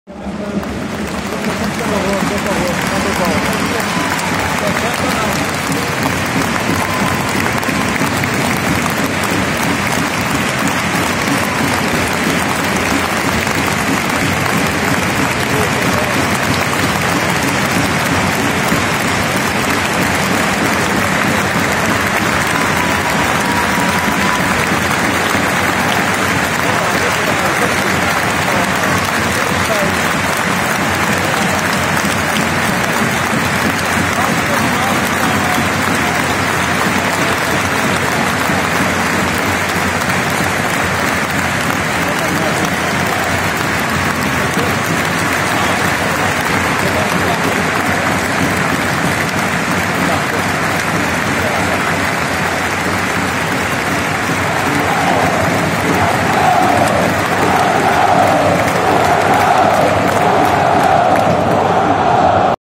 Homenagem a Fernando Gomes, Bibota
Minuto 9 no Dragão de homenagem a Fernando Gomes.